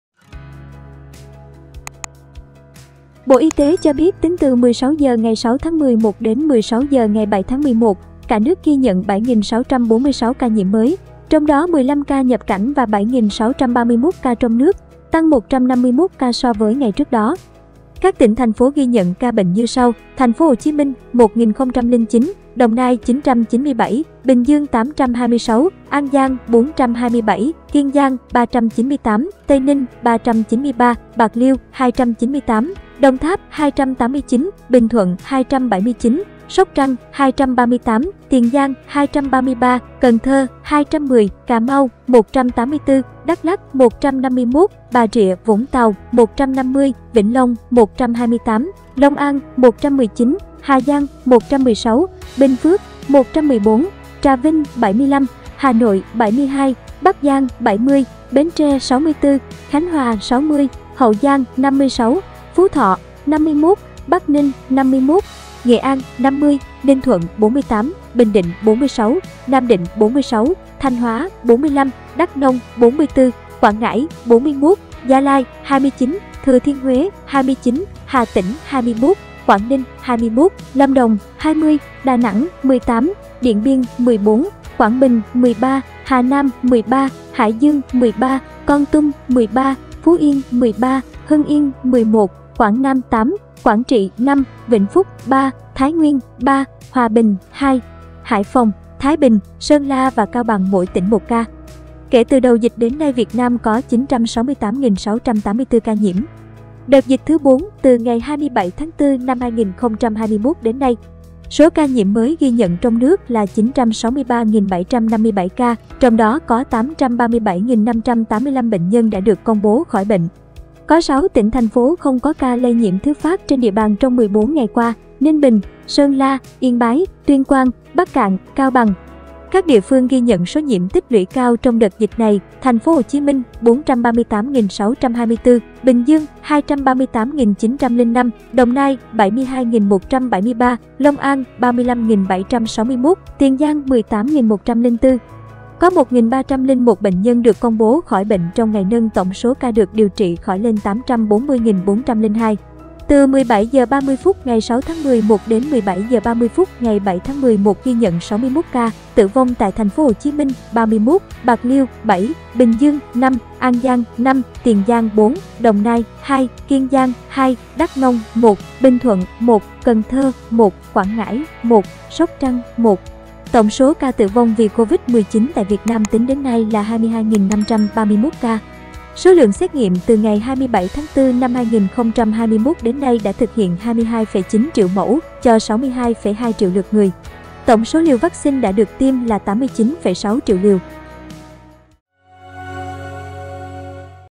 Tối 7-11: Cả nước thêm 7.646 ca COVID-19 mới, TP.HCM có 1.009 ca
Trong 24 giờ qua, có thêm 7.646 ca nhiễm mới được ghi nhận ở 56 tỉnh, thành phố. Số ca mới tăng mạnh tại Tây Ninh, Bình Phước và Đồng Tháp. Tại TP.HCM, có thêm 1.009 ca dương tính.